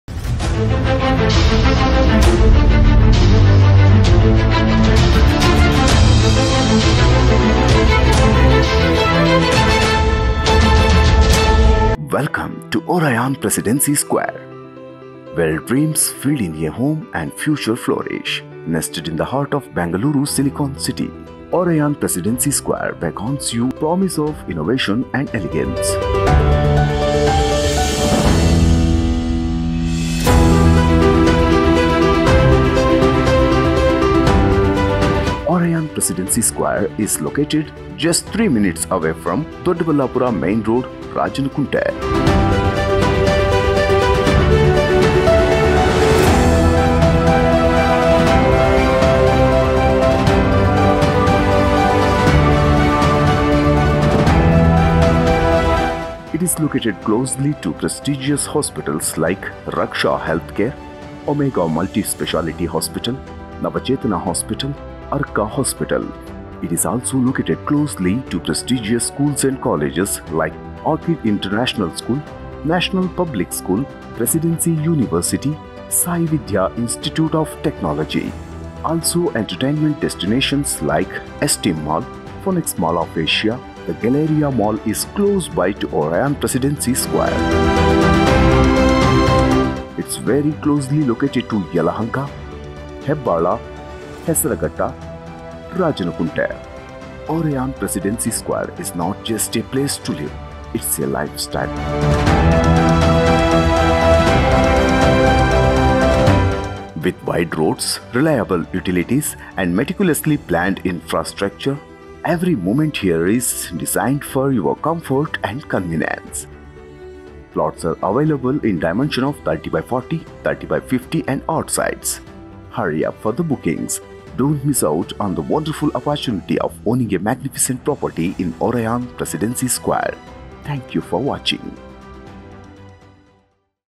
Discover Oraiyan Presidency Square: Affordable Luxury Villa Plots in Yelahanka🌟
Oraiyan Presidency Square is your gateway to premium living near Rajankunte, Yelahanka. Here’s what sets us apart:

🏡 Ready for Registration & Construction: Move in seamlessly with all approvals in place.
📜 DC Conversion & E-Khata: Hassle-free ownership with cleared legalities.
💰 Affordable Pricing: Luxurious villa plots start at just ₹2650 per sqft.
📈 High Investment Returns: Previous projects have doubled in value, offering great growth potential.
📍 Convenient Location: Close to top schools, hospitals, and entertainment hubs, all within 15 minutes.
🏗️ Flexible Options: Build your dream villa or choose from pre-designed options.
💵 Easy Financing: Loans available with interest rates starting at 8.5%.
🛠️ Excellent Amenities: Cement roads, individual water connections, and luxury amenities for comfortable living.